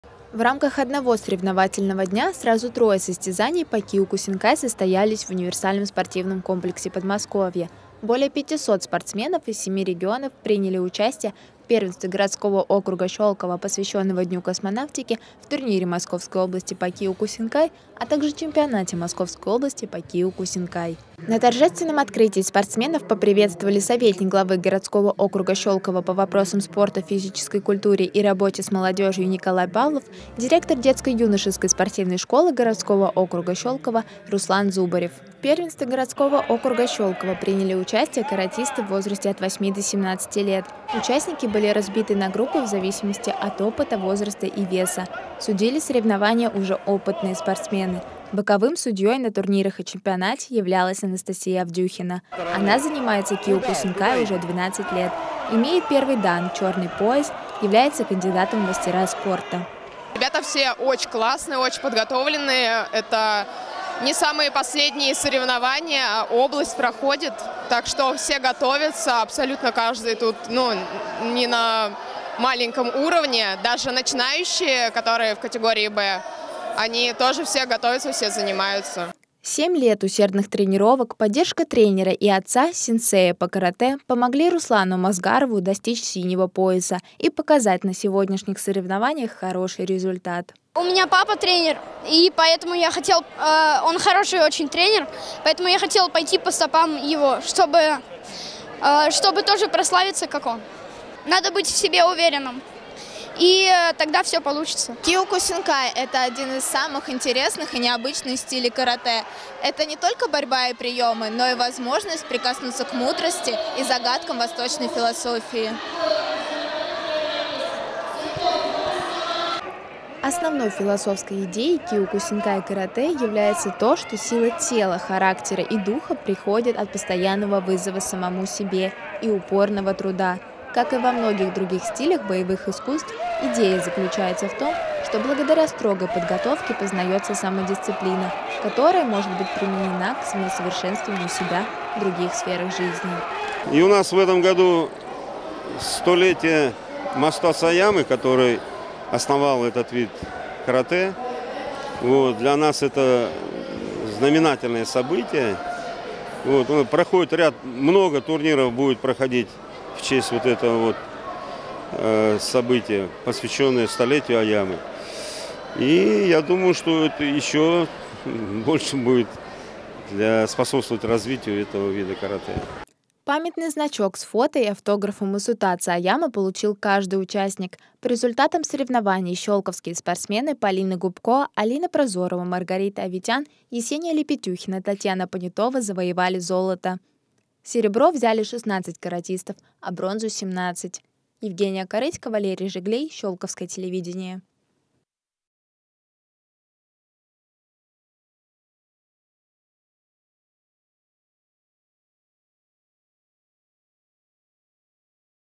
Трое состязаний по киокусинкай в рамках одного соревновательного дня прошли в УСК «Подмосковье»
Каратисты из 28 регионов России встретились в УСК «Подмосковье», чтобы определить лучшего в поединках в Первенстве г. о. Щёлково, посвящённого Дню Космонавтики, в турнире Московской области по киокусинкай, а также чемпионате Московской области.